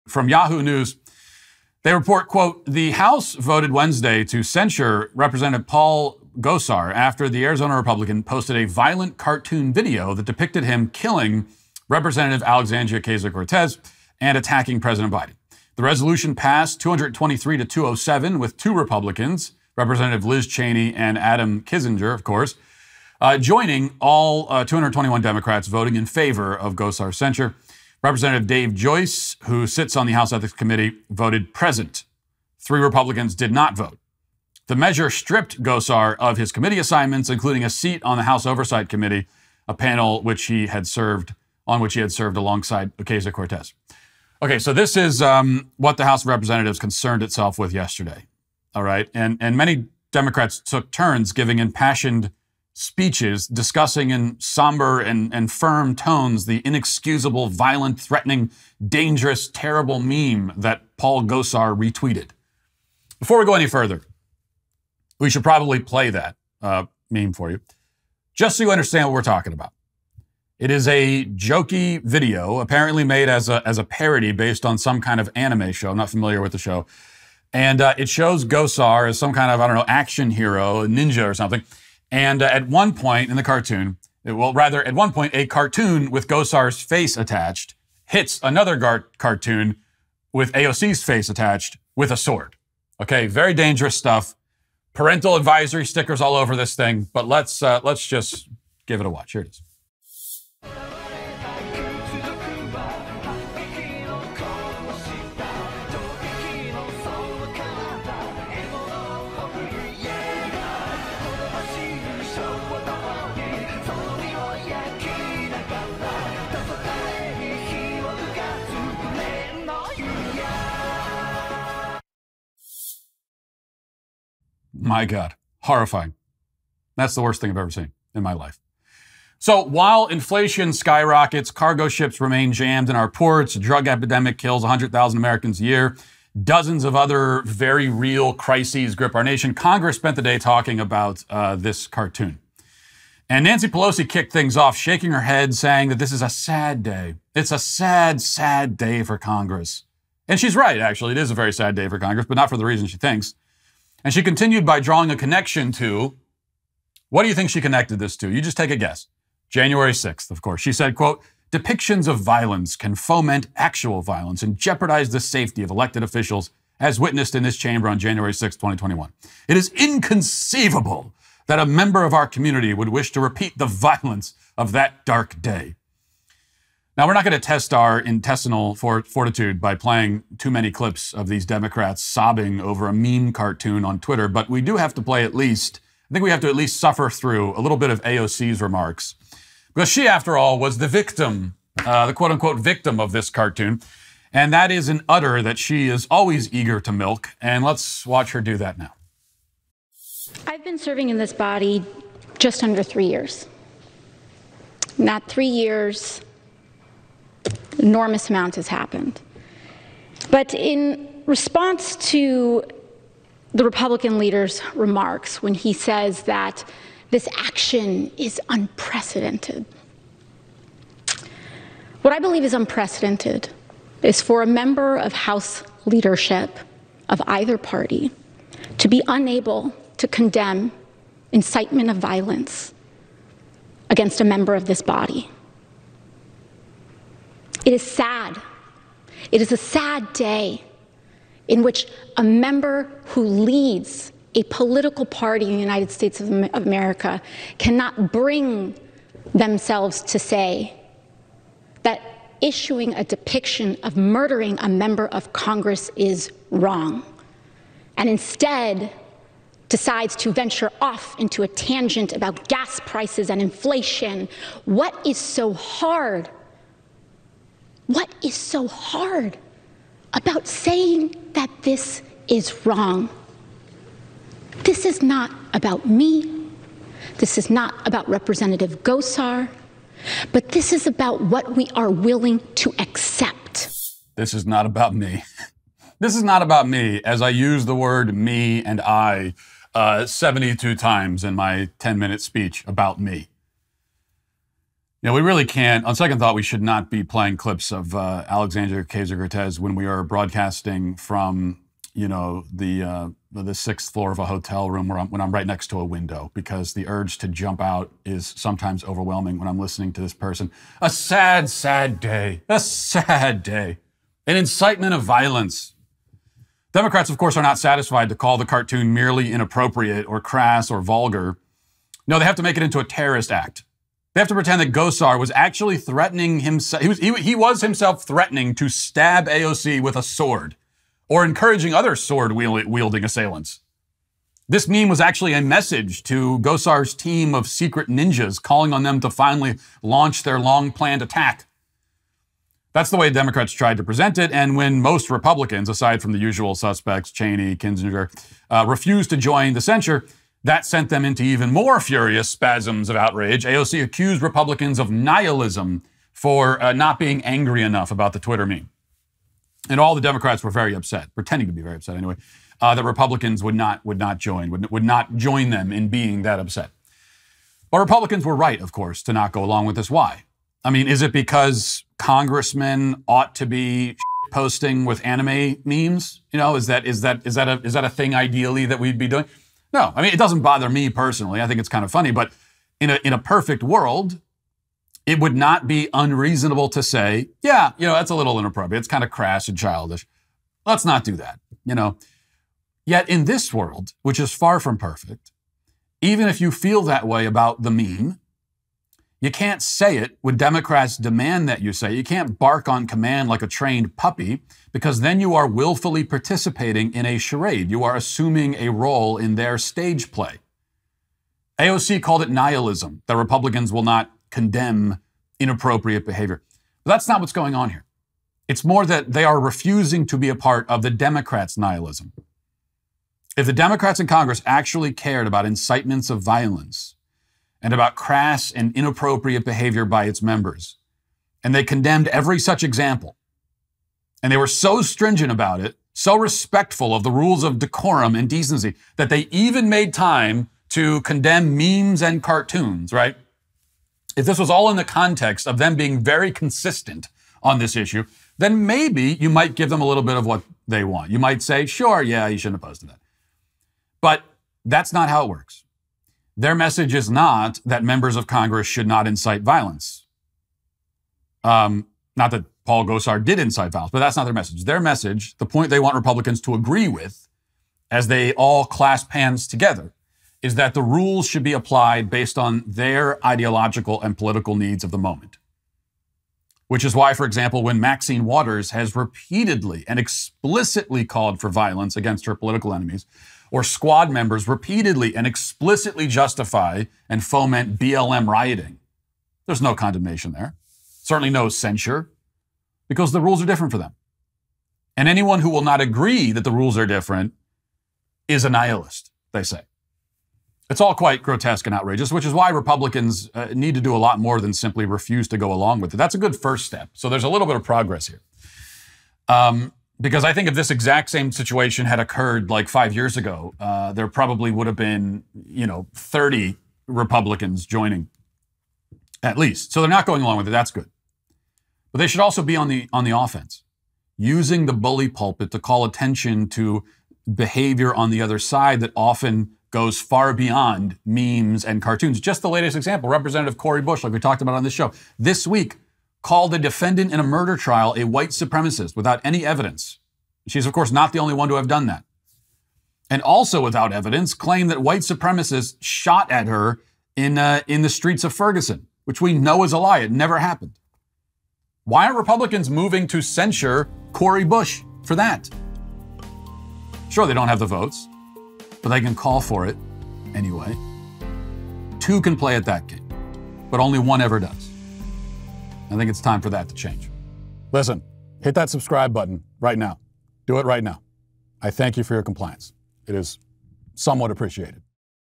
Democrats TRIGGERED Over Meme Video Made By Republican Representative
After Rep. Paul Gosar posted a meme on Twitter that triggered leftists everywhere — he was censured by the house for it. Why are the Dems triggered over a cartoon?

Joe Biden is trying to crack down on American freedom with his tyrannical vaccine mandate. We’re not complying, and neither should you.